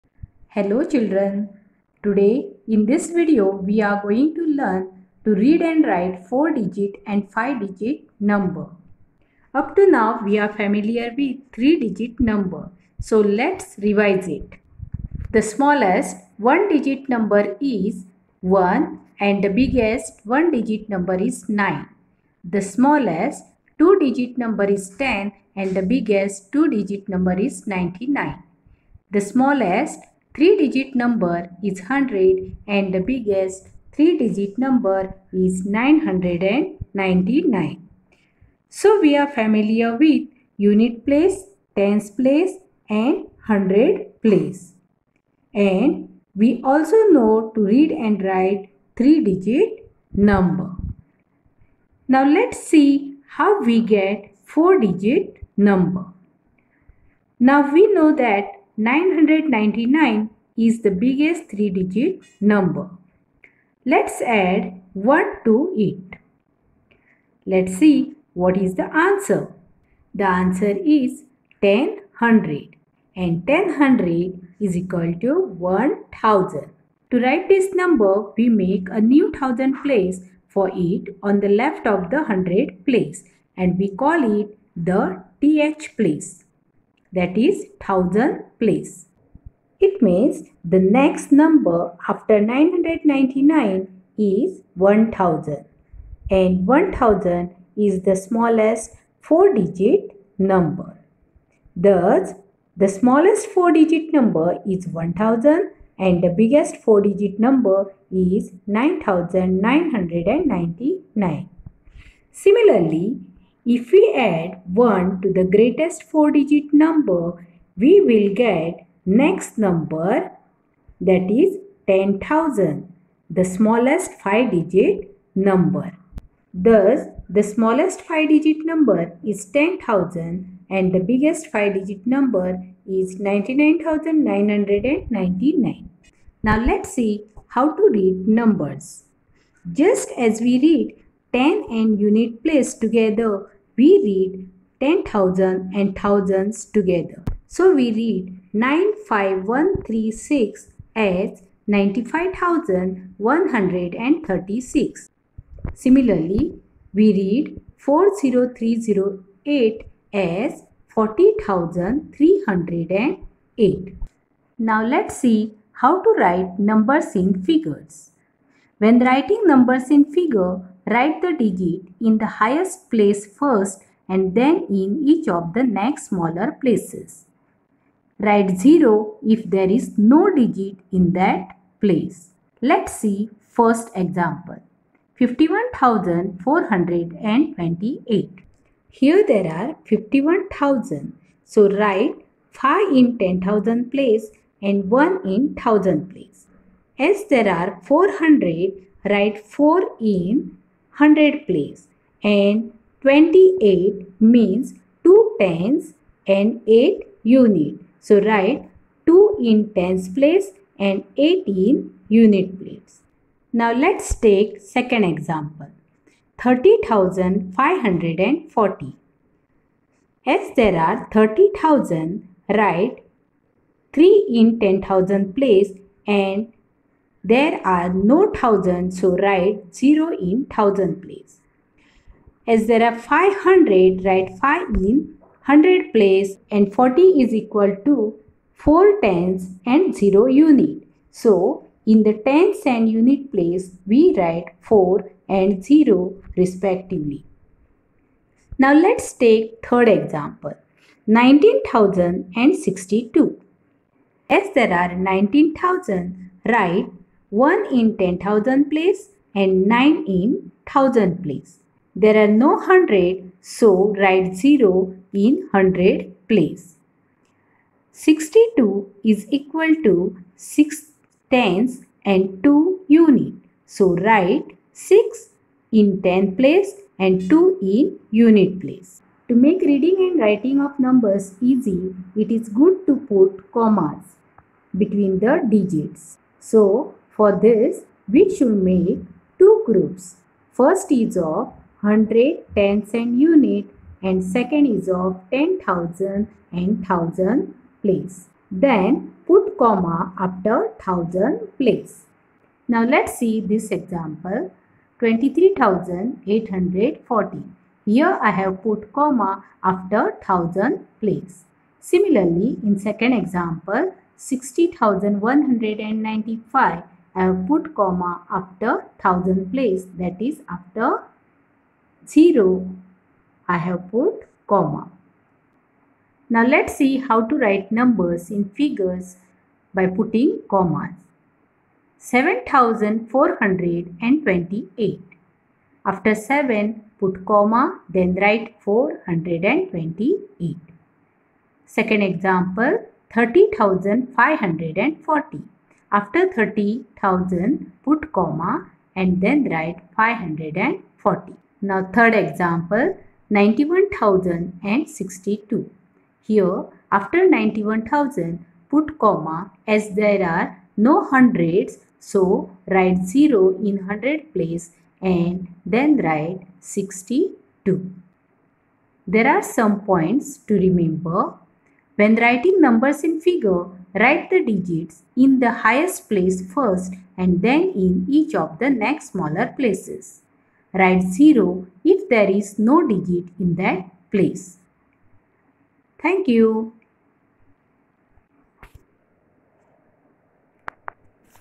Introducing 4 & 5 digit number/Std:4th/English medium/2.Number work
Maharashtra state

Mathematics
Standard Four
Part One
2.Number Work

Explanation by
Mrs. Prerana Bhosale